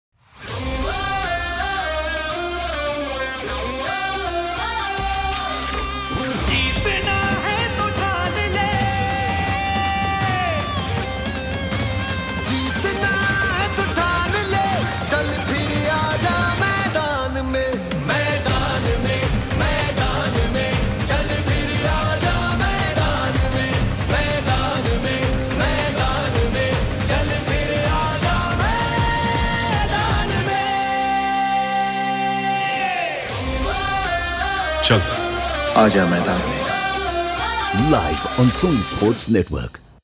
𝗕𝗥𝗜𝗡𝗚𝗜𝗡𝗚 𝗧𝗘𝗡𝗡𝗜𝗦 𝗧𝗢 𝗘𝗩𝗘𝗥𝗬 𝗛𝗢𝗠𝗘 𝗜𝗡 𝗜𝗡𝗗𝗜𝗔 - Tennis Premier League 2023 Powered by Clear Premium Water
Tennis Premier League with the most exciting 20-point format is back with top players ready to serve up thrilling action from December 1️⃣2️⃣th to 1️⃣7️⃣th! 📆📢

📲👀 Let's come together to support our favorite teams and make season 5 unforgettable.

🙌🎾 Don't forget to catch all the action live on @SonySportsNetwork, the home of Indian Tennis! 📺🔥

This is where the Legends are made! This is where the Legends are made! TPLSeason5 Powered by @clearpremiumwater7102